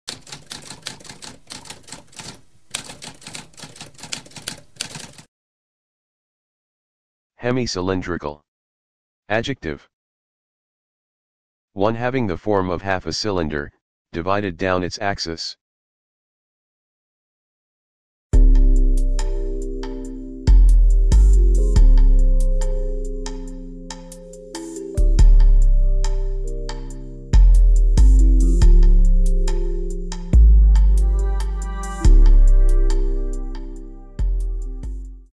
What does hemicylindrical mean?
A spoken definition of hemicylindrical.

Intro Sound:
Typewriter - Tamskp